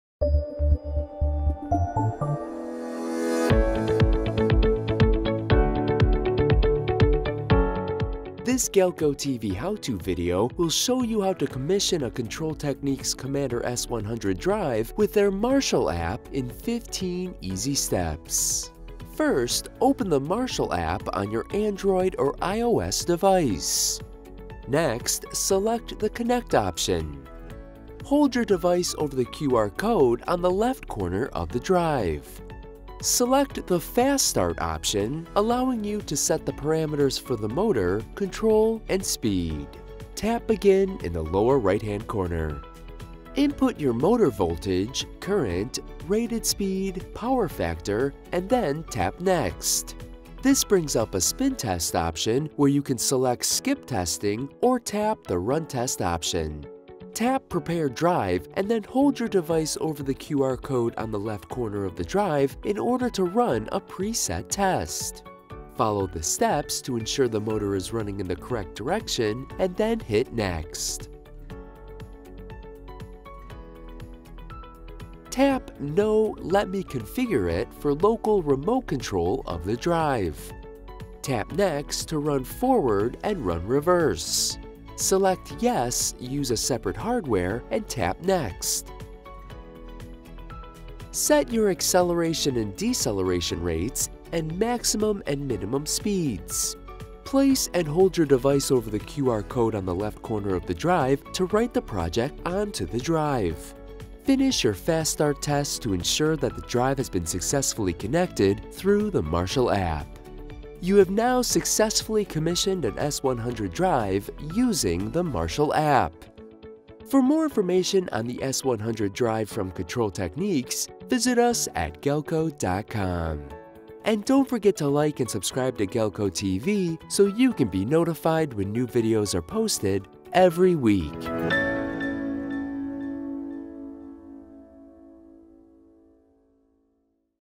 How To Set Up Control Techniques Commander S100 Drive With the MARSHAL App | Galco
This GalcoTV How To video will show you how to commission the Control Techniques Commander S100 Drive with their Marshal app in 15 easy steps:

1. Open the Marshal app on your Android or iOS device.
2. Select the Connect Option.
3. Hold your device over the QR code on the left corner of the drive.
4. Select the FastStart Option, allowing you to set the parameters for the Motor, Control, and Speed.
5. Tap begin in the lower right corner.

Motor

6. Input your Motor Voltage, Current, Rated Speed, Power Factor, and then tap next.
7. This brings up a Spin Test option where you can select Skip Testing or tap the Run Test Option.
8. Tap Prepare Drive and then hold your device over the QR code on the left corner of the drive in order to run a preset test.
9. Follow the steps to ensure the motor is running the correct direction and then hit next.

Control

10. Tap "No, Let Me Configure It" for Local/Remote control of the drive.
11. Tap Next to Run Forward + Run Reverse.
12. Select "Yes, Use a Separate Hardware" and tap Next.

Speed
13. Set your acceleration/deceleration rates, and maximum/minimum speeds.
14. Place and hold your device over the QR code in the left corner of the drive to write the project onto the drive.
15. Finish your FastStart test to ensure that the drive has been successfully connected through the Marshal app.

Sign up for Galco’s newsletter that features our newest products, our can't-miss deals, our best videos, and total access to Galco’s electronics expertise